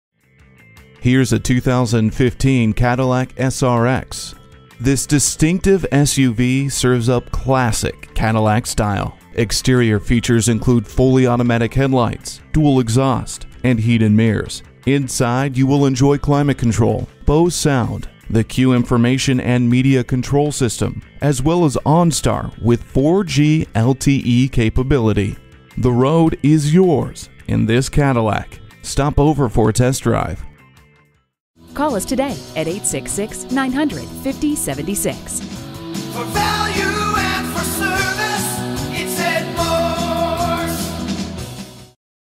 Used 2015 Cadillac SRX Tampa FL St. Petersburg, FL 189034A - SOLD
Year: 2015
Make: Cadillac
Model: SRX
Trim: Performance Collection
Engine: 3.6l 6 Cylinder Not Specified
Transmission: FWD 6-Speed Automatic
Mileage: 14847
Stock #: 189034A
VIN: 3GYFNCE36FS557250

Address:
101 East Fletcher Avenue